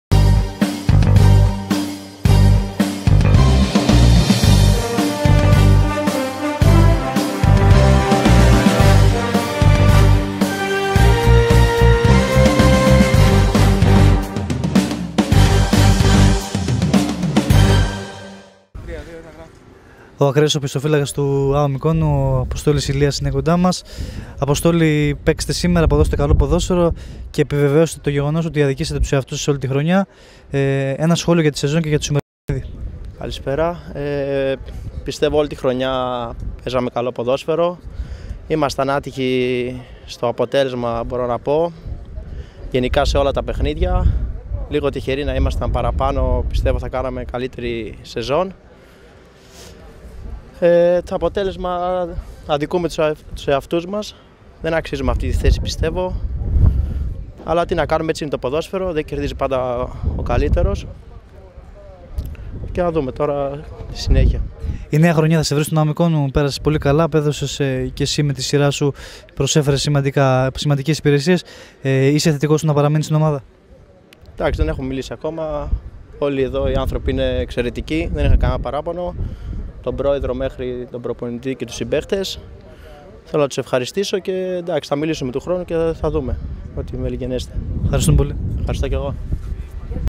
Ηλίας: Μας έλειψε η τύχη τη φετινή χρονιά
Ο ποδοσφαιριστής του Α.Ο. Μυκόνου μιλάει στη κάμερα του sportcyclades.gr και στον Κυριάκο Παντζαρίδη για το ματς της ομάδας του με τον Α.Ο. Σύρου καθώς και για την φετινή χρονιά.